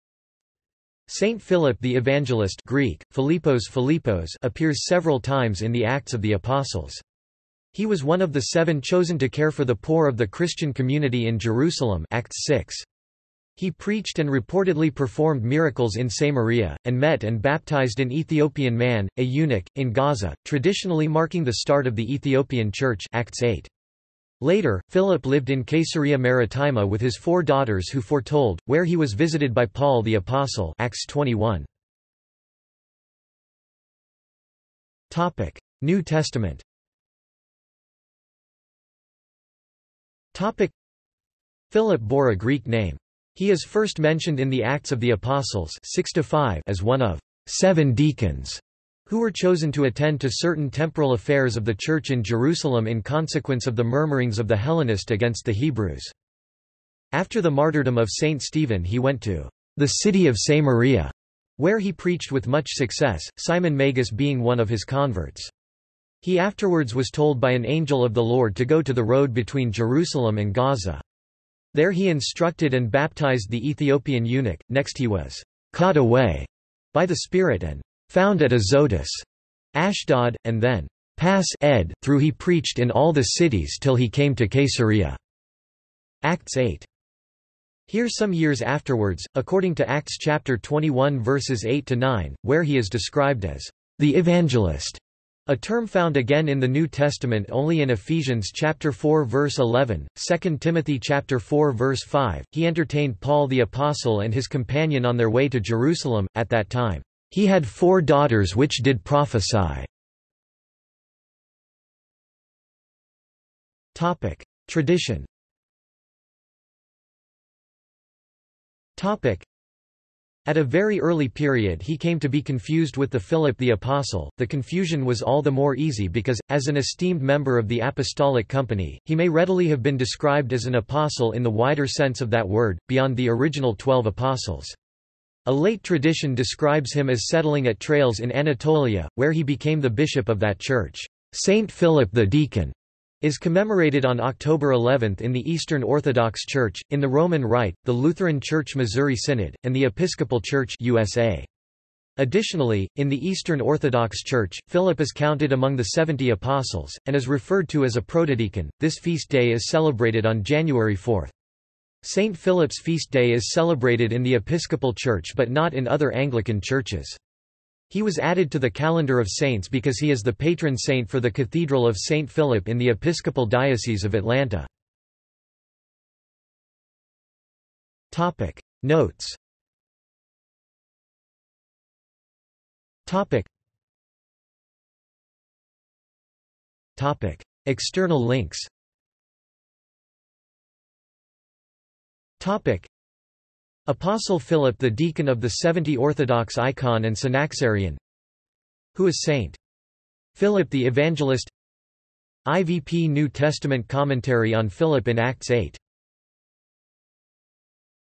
Philip the Evangelist | Wikipedia audio article
This is an audio version of the Wikipedia Article:
Philip the Evangelist

SUMMARY
Saint Philip the Evangelist (Greek: Φίλιππος, Philippos) appears several times in the Acts of the Apostles.  He was one of the Seven chosen to care for the poor of the Christian community in Jerusalem (Acts 6). He preached and reportedly performed miracles in Samaria, and met and baptised an Ethiopian man, a eunuch, in Gaza, traditionally marking the start of the Ethiopian Church (Acts 8). Later, Philip lived in Caesarea Maritima with his four daughters who foretold, where he was visited by Paul the Apostle (Acts 21).